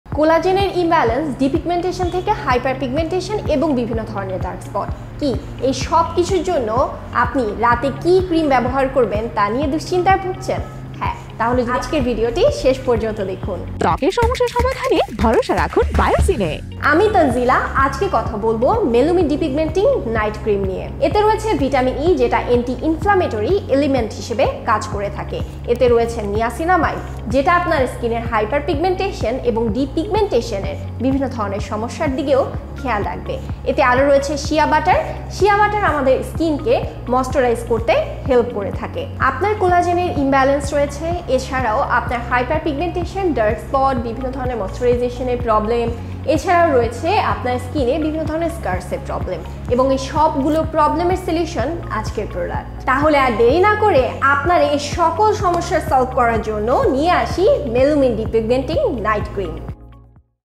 Depigmenting Night Cream - রাতভর চলুক স্কিন রিপেয়ারিং!
রোদে পোড়া দাগ, বয়সের ছাপ, মেছতা ও স্কিনের অনুজ্জ্বল ভাব - এসব দাগ দূর করে স্কিনকে উজ্জ্বল করতে আছে Dermedic এর Melumin Depigmenting Night Cream. যা আপনার স্কিনের সকল দাগ দূর করে এবং নতুনভাবে দাগ পড়া থেকে স্কিনকে রাখে সুরক্ষিত।

স্কিনের মৃত কোষ সরিয়ে নতুন ত্বক প্রতিস্থাপনের সাহায্য করে এই ক্রিম। যার ফলে স্কিনের অনুজ্জ্বল ও নিস্তেজ ভাব দূর হয় এবং স্কিন থাকে তারুণ্যময়, উজ্জ্বল ও প্রাণবন্ত।

অর্ডার করতে এখনই ভিজিট করুন ➡️ অথবা কল করুন 📲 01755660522 কিংবা ইনবক্স করুন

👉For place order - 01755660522